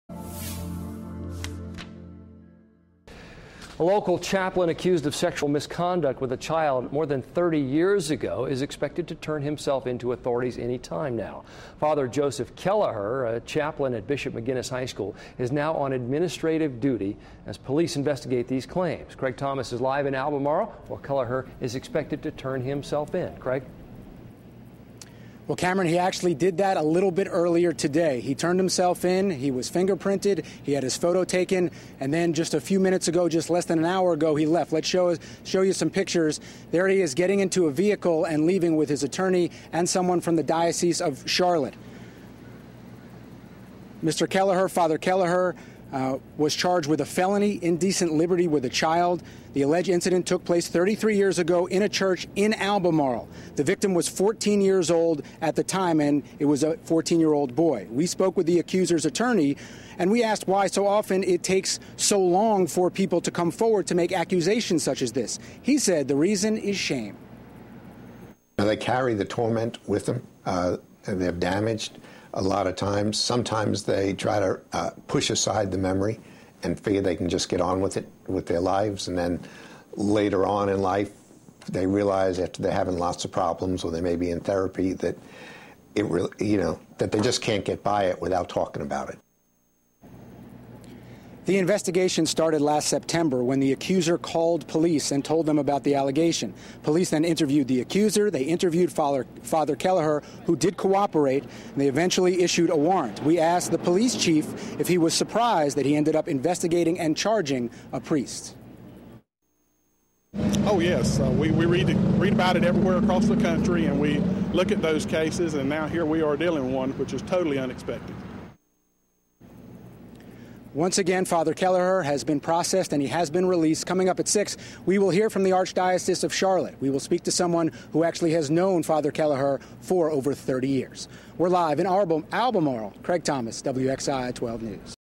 Local Priest Surrenders To Authorities
The chaplain at Winston-Salem Bishop McGuinness surrendered to authorities in Albermarle Thursday afternoon.